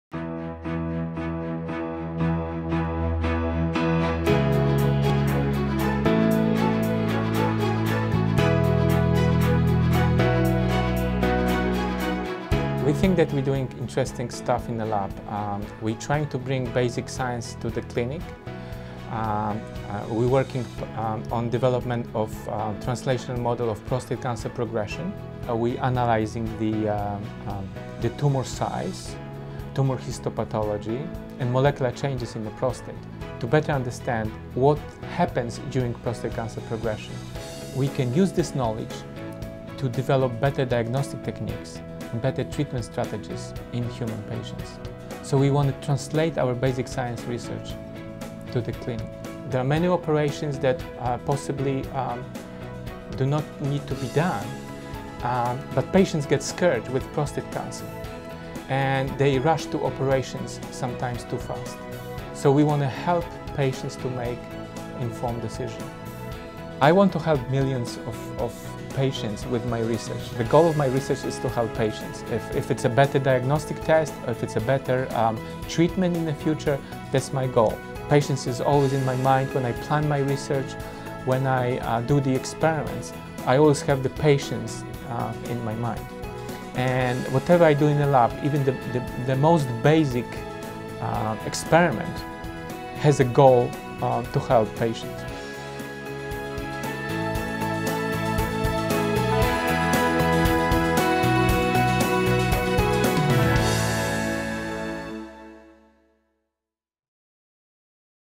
Upstate Medical University-"Research Matters"-Leszek Kotula, MD/PhD
Dr Kotula explains how basic science research in his lab at Upstate Medical University in Syracuse, NY will benefit prostate cancer patients with better diagnostic and treatment options.